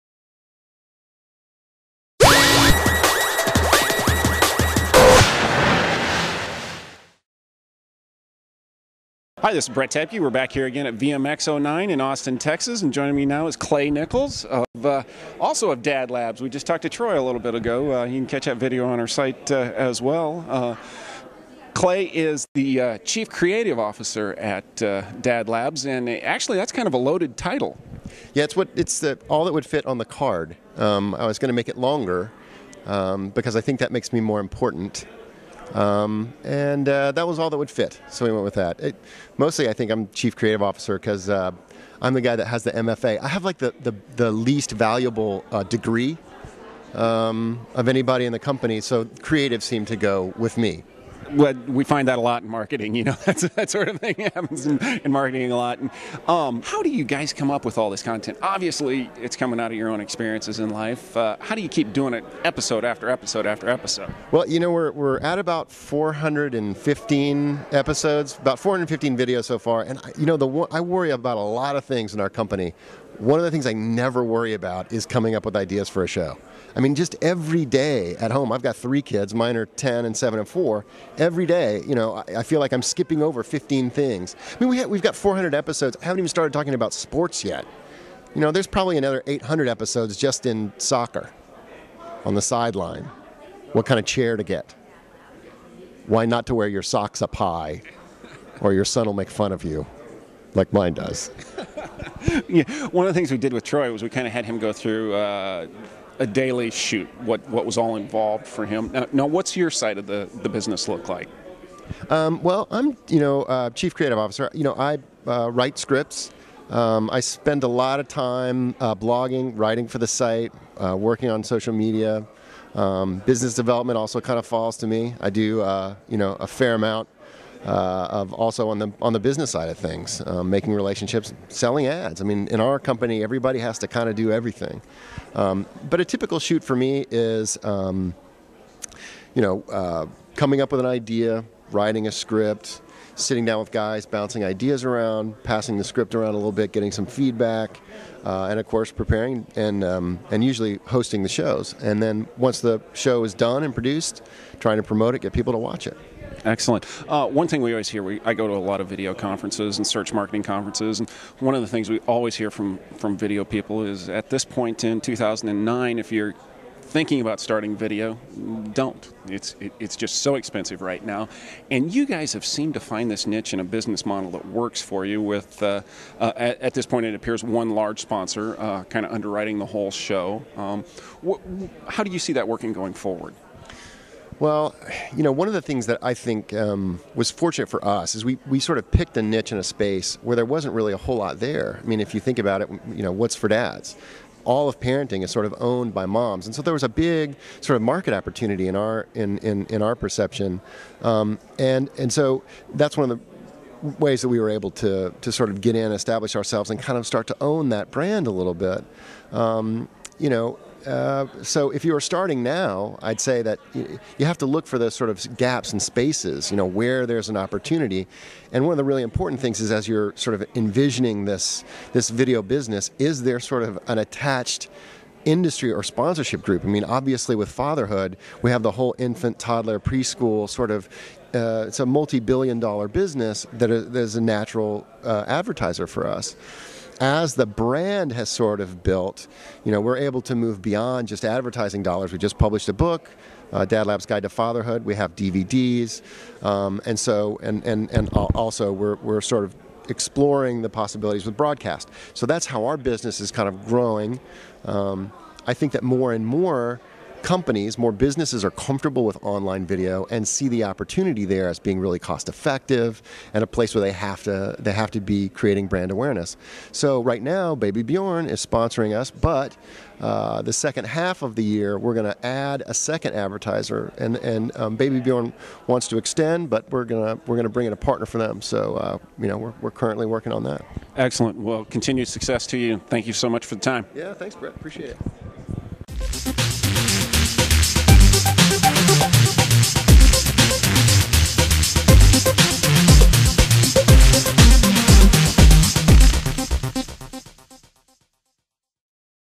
DadLabs CCO and Co-founder, Clay Nichols at VMX 2009
WebmasterWorld's Brett Tabke talks to Clay Nichols, the Chief Creative Officer at DadLabs, about some of the challenges of running a company that produces Web video and the importance of building a brand.